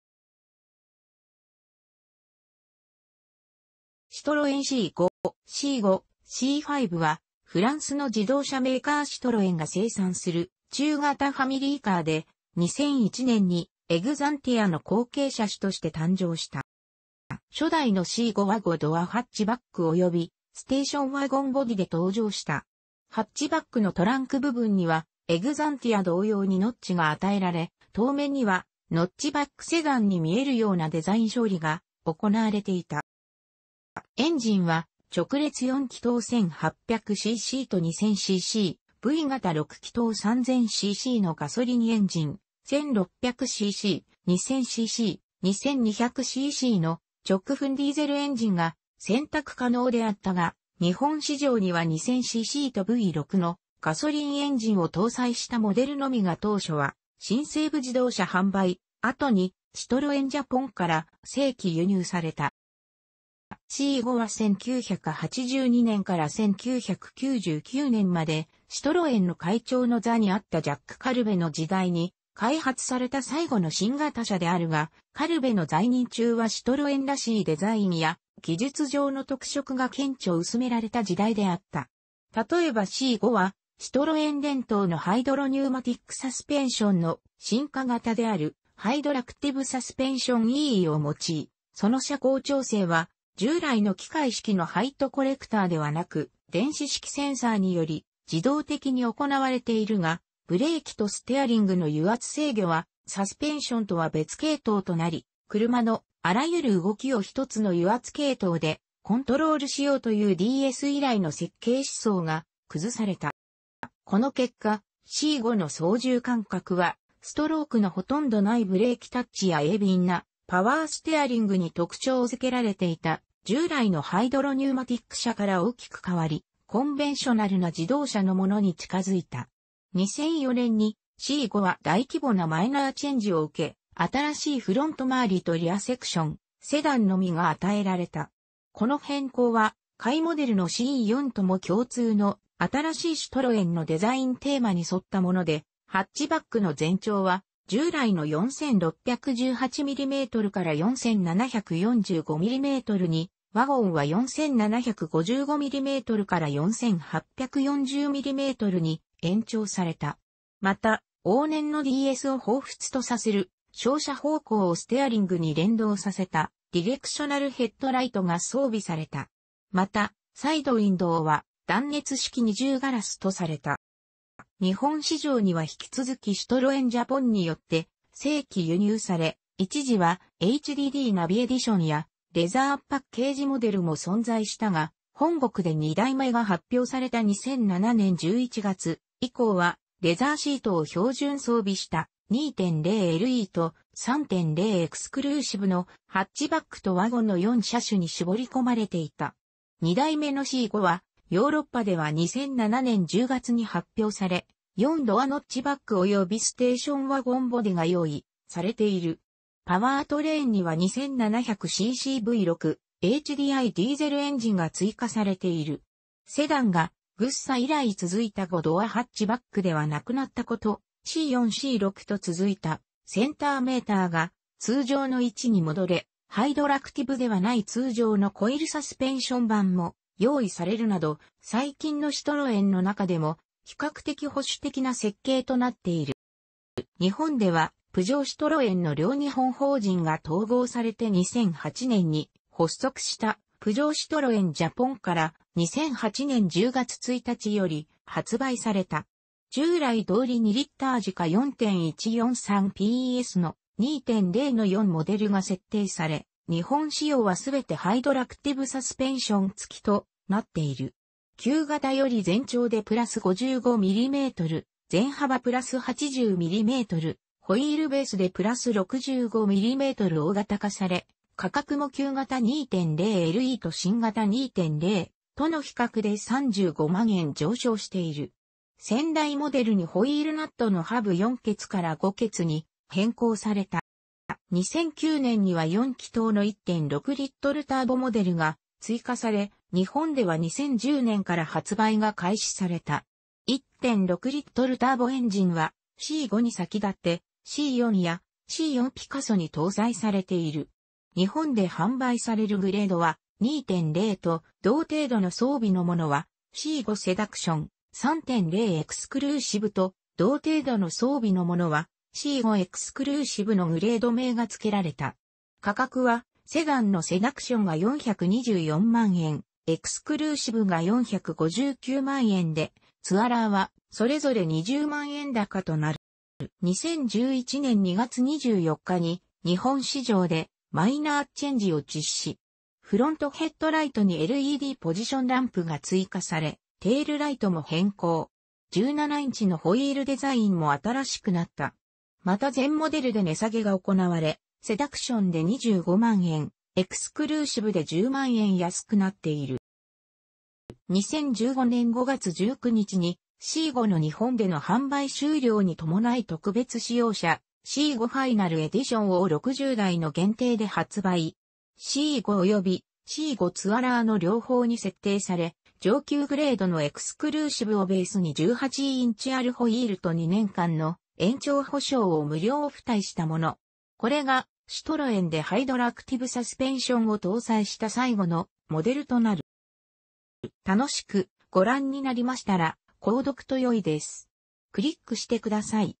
シトロエン・C5
C5（シーファイブ）はフランスの自動車メーカーシトロエンが生産する中型ファミリーカーで、2001年にエグザンティアの後継車種として誕生した。

初代のC5は5ドアハッチバックおよびステーションワゴンボディで登場した。ハッチバックのトランク部分にはエグザンティア同様にノッチが与えられ、遠目にはノッチバックセダンに見えるようなデザイン処理が行われていた。

エンジンは直列4気筒1,800ccと2,000cc、V型6気筒3,000ccのガソリンエンジン、1,600cc、2,000cc、2,200ccの直噴ディーゼルエンジンが選択可能であったが、日本市場には2000ccとV6のガソリンエンジンを搭載したモデルのみが当初は新西武自動車販売、後にシトロエン・ジャポンから正規輸入された。

C5は1982年から1999年までシトロエンの会長の座にあったジャック・カルヴェの時代に開発された最後の新型車であるが、カルヴェの在任中はシトロエンらしいデザインや技術上の特色が顕著に薄められた時代であった。例えばC5はシトロエン伝統のハイドロニューマティックサスペンションの進化型であるハイドラクティブサスペンションIIIを用い、その車高調整は従来の機械式のハイトコレクターではなく電子式センサーにより自動的に行われているが、ブレーキとステアリングの油圧制御はサスペンションとは別系統となり、車のあらゆる動きを1つの油圧系統でコントロールしようというDS以来の設計思想が崩された。

この結果、C5の操縦感覚はストロークのほとんどないブレーキタッチや鋭敏なパワーステアリングに特徴付けられていた従来のハイドロニューマティック車から大きく変わり、コンベンショナルな自動車のものに近付いた。
2004年にC5は大規模なマイナーチェンジを受け、新しいフロント周りとリアセクション（セダンのみ）が与えられた。この変更は下位モデルの C4とも共通の、新しいシトロエンのデザインテーマに沿ったもので、ハッチバックの全長は従来の4,618mmから4,745mmに、ワゴンは4,755mmから4,840mmに延長された。また、往年のDSを髣髴とさせる照射方向をステアリングに連動させた「ディレクショナル・ヘッドライト」が装備された。また、サイドウインドウは断熱式二重ガラスとされた。

日本市場には引き続きシトロエン・ジャポンによって正規輸入され、一時はHDDナビエディションやレザーパッケージモデルも存在したが、本国で2代目が発表された2007年11月以降は、レザーシートを標準装備した「2.0LE」と「3.0エクスクルーシブ」のハッチバックとワゴンの4車種に絞り込まれていた。
2代目のC5はヨーロッパでは2007年10月に発表され、4ドアノッチバック及びステーションワゴン(ツアラー)ボディが用意されている。パワートレーンには2,700ccV6HDI ディーゼルエンジンが追加されている。セダンがGSA以来続いた5ドアハッチバックではなくなったこと、C4・C6と続いたセンターメーターが通常の位置に戻され、ハイドラクティブではない通常のコイルサスペンション版も用意されるなど、最近のシトロエンの中でも比較的保守的な設計となっている。

日本ではプジョー・シトロエンの両日本法人が統合されて2008年に発足したプジョー・シトロエン・ジャポンから、2008年10月1日より発売された。従来通り2リッター直4・143PSの「2.0」(399.0万円・ツアラー419.0万円）と、3リッターV6・215PSの「3.0エクスクルーシブ」(479.0万円・ツアラー499.0万円)の4モデルが設定され、日本仕様は全てハイドラクティブサスペンション付きとなっている。旧型より全長で+55mm、全幅+80mm、ホイールベースで+65mm大型化され、価格も旧型2.0LEと新型2.0との比較で35万円上昇している。先代モデルにホイールナットのハブ4穴から5穴に変更された。

2009年には4気筒の1.6Lターボモデルが追加され、日本では2010年から発売が開始された。1.6Lターボエンジンは、C5に先立ってC4やC4ピカソに搭載されている。日本で販売されるグレードは、2.0と同程度の装備のものは「C5セダクション」、3.0エクスクルーシブと同程度の装備のものは「C5エクスクルーシブ」のグレード名がつけられた。価格はセダンのセダクションが424万円、エクスクルーシブが459万円で、ツアラーはそ...